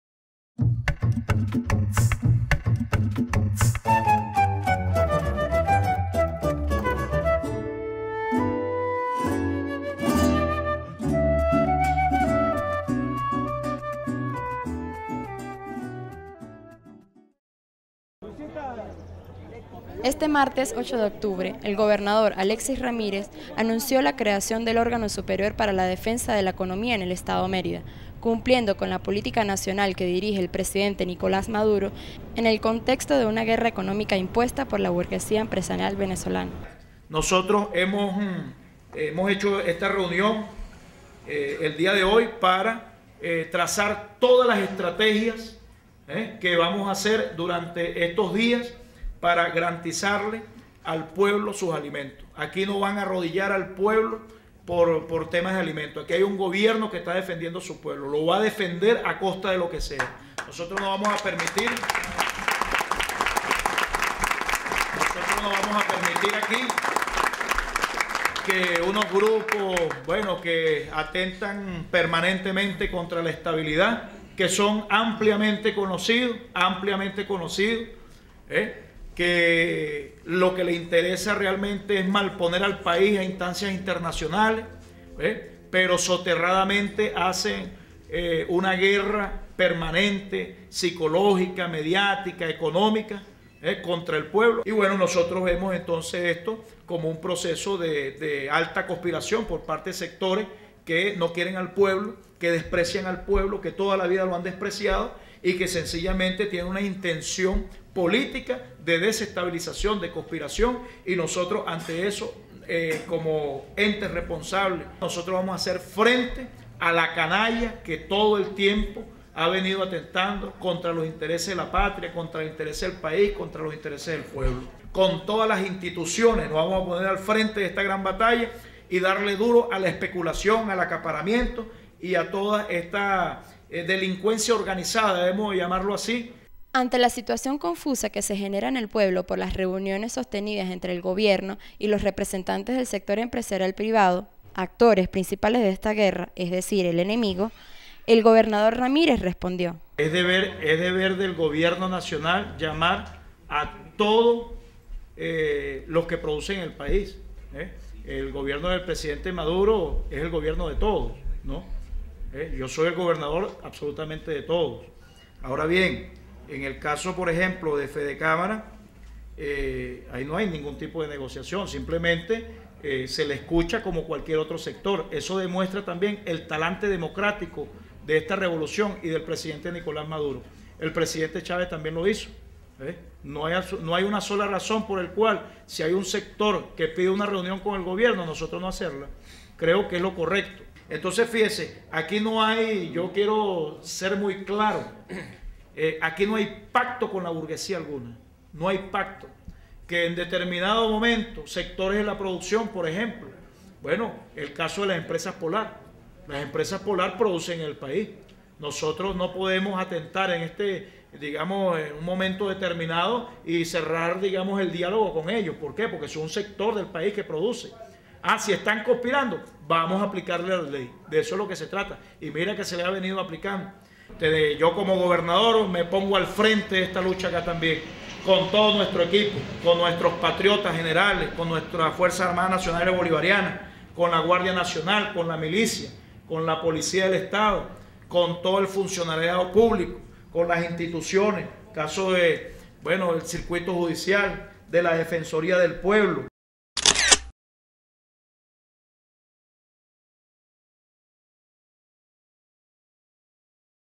Creado Órgano Superior para la Defensa de la Economía del Estado Mérida.
Este Martes 8 de Octubre El Gobernador Aléxis Ramírez. Anunció la creación del órgano superior para la defensa  de la economía en el estado Mérida, cumpliendo con la  política nacional que dirige el Presidente Nicolás Maduro en el contexto de una guerra económica impuesta por la burguesía empresarial venezolana.